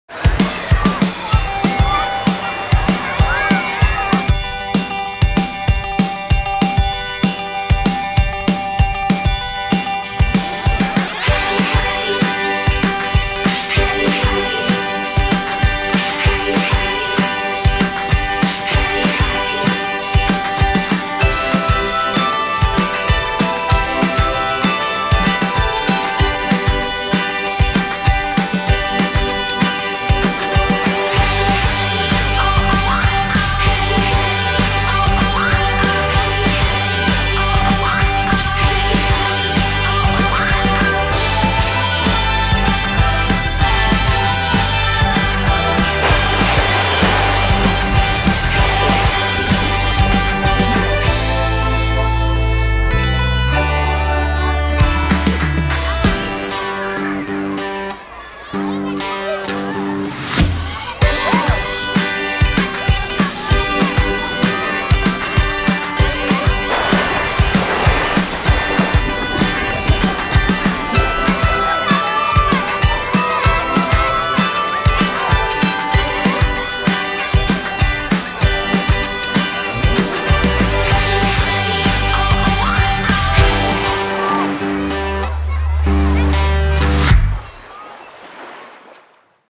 A l'estiu també mengem bé!
Espot promocional de la campanya dalimentació infantil a l'estiu de la Creu Roja a Catalunya. Aquest estiu, més de 3.000 infants (unes 1.600 famílies) rebran ajuda alimentària de la entitat humanitària, garantint una alimentació sana i equiliobrada fora del període escolar.
Els petits protagonistes d'aquest espot són fills de voluntariat i tècnics de la Creu Roja. Gràcies a tots!